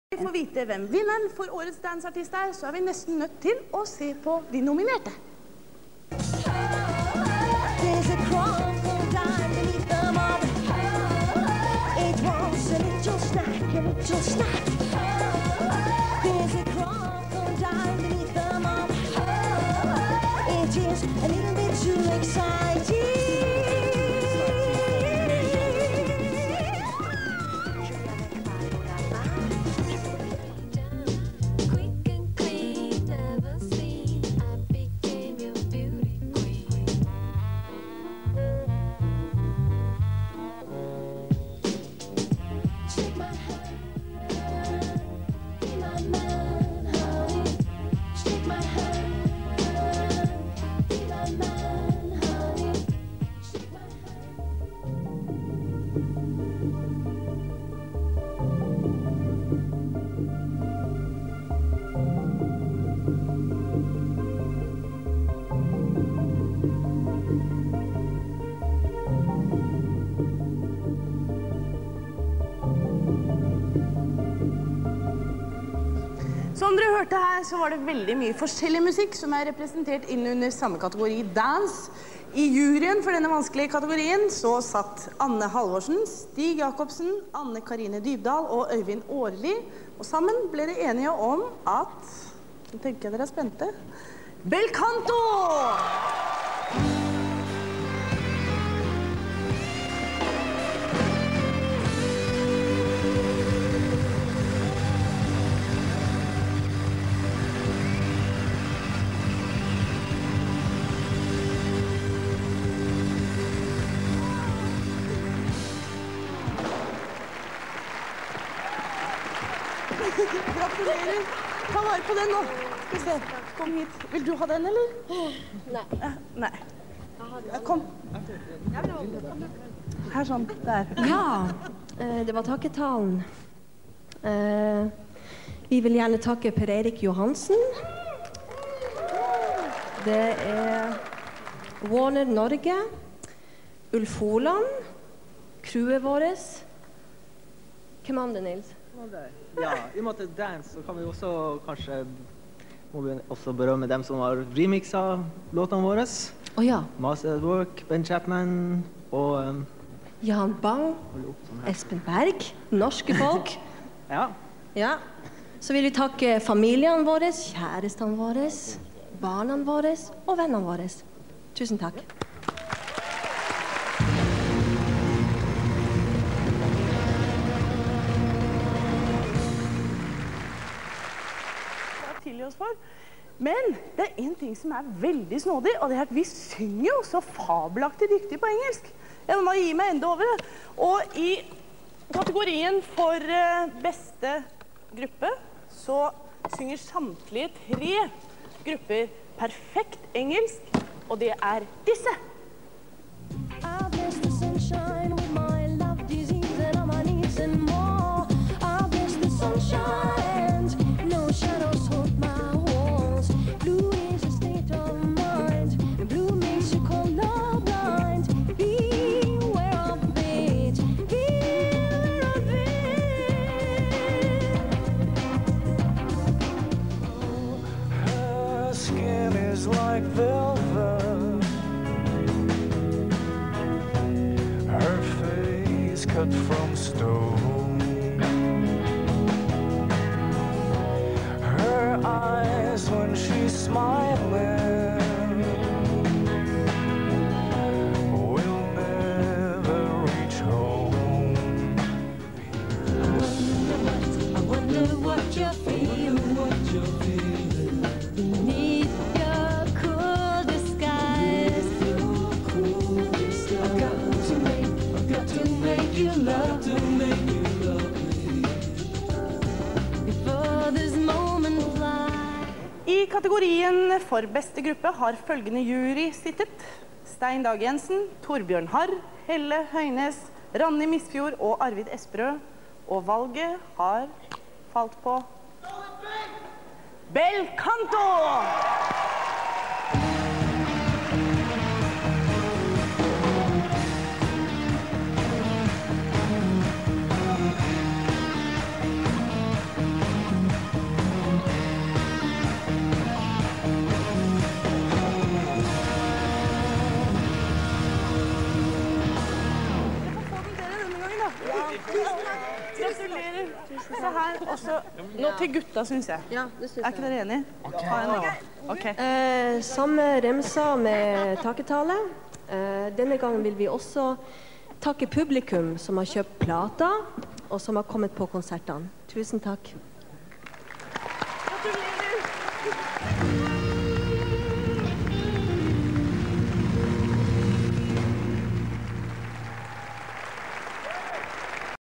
Bel Canto - winner of Spellemansprisen 1997
Bel Canto won the award in two categories - Best Dance and Best Band.